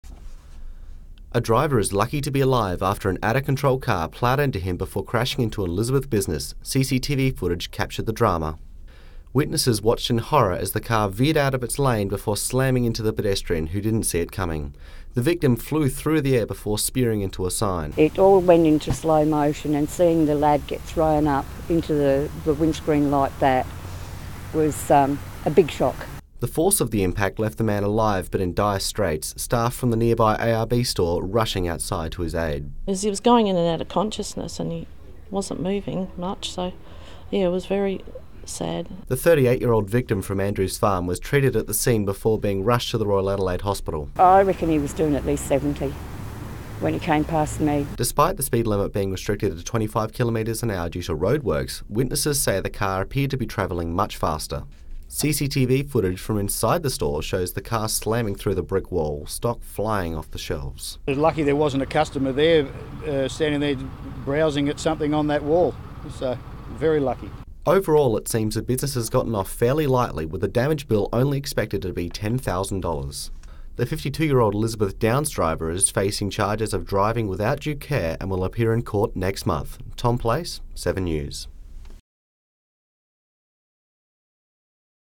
Channel Seven Pedestrian hit package - footage lost.
While completing work experience with Channel 7 I was able to write up a  story of my own and shoot a piece to camera. Unfortunately when I went to edit the story almost all the footage was lost due to a server issue they were experiencing.

The video is voiced and edited in house at Seven.